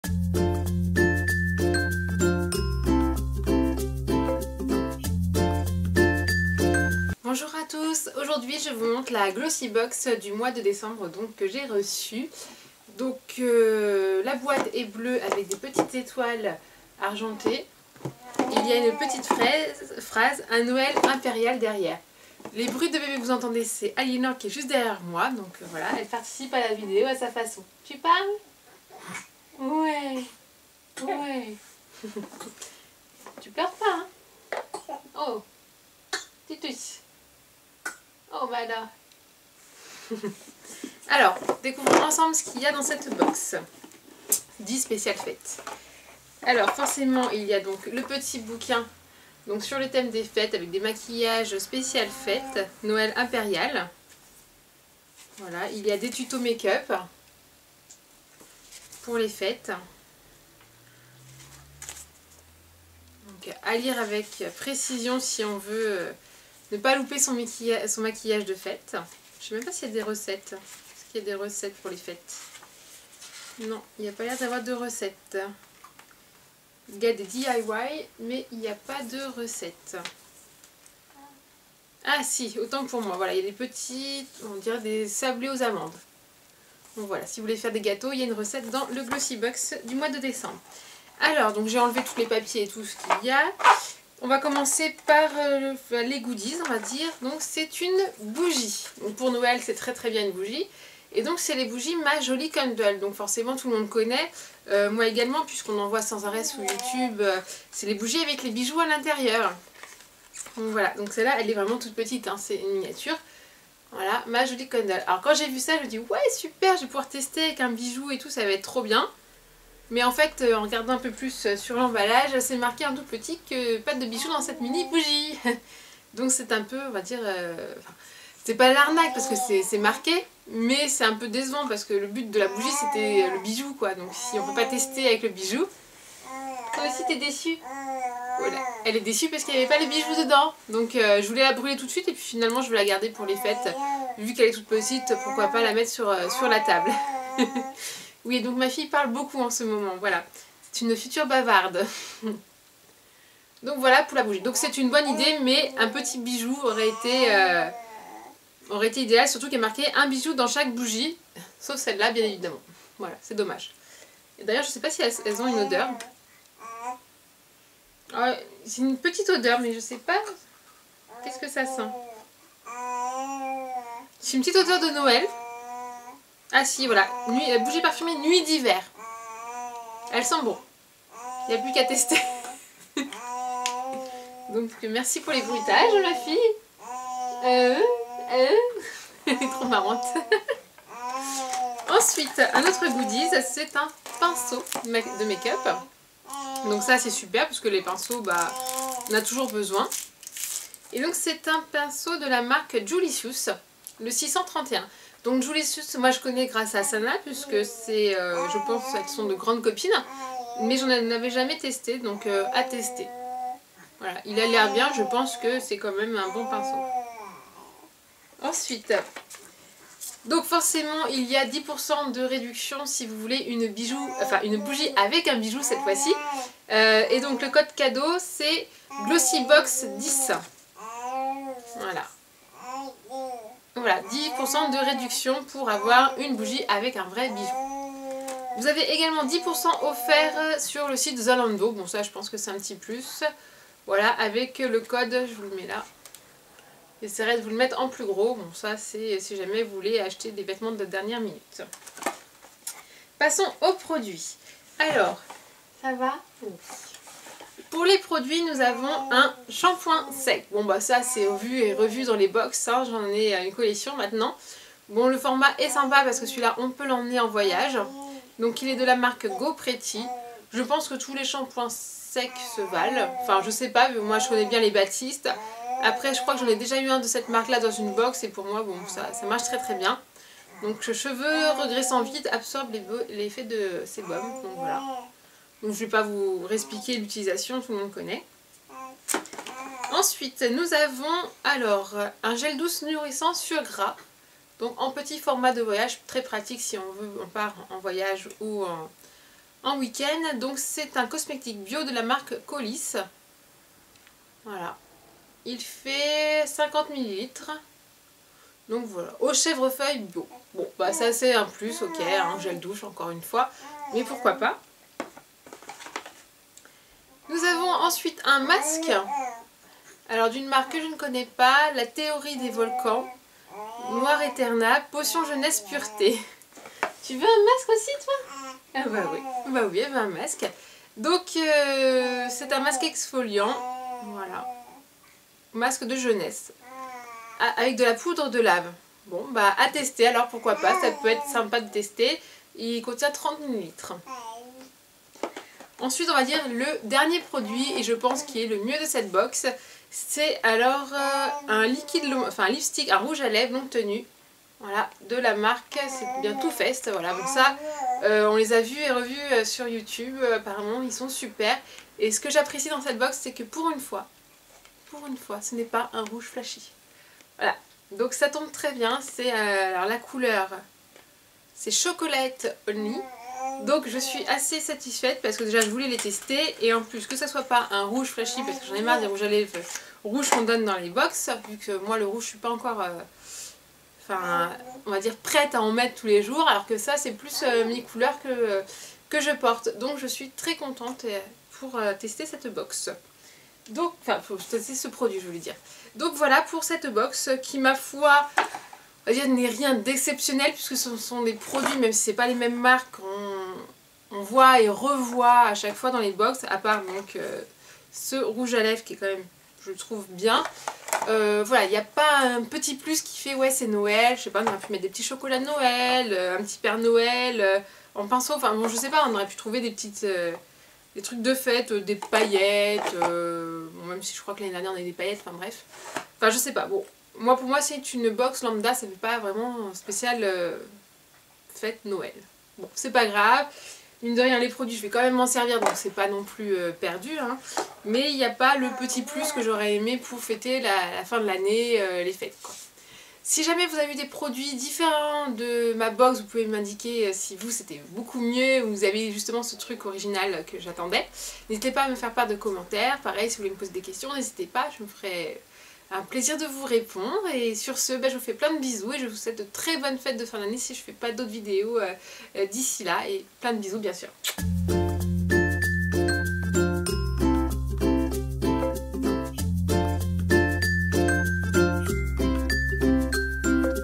♥Glossy box décembre♥
♥Dans cette vidéo, on découvre le contenu de la Glossybox du mois de décembre. ♥

Je vous souhaite à tous de très bonnes fêtes de fin d'année.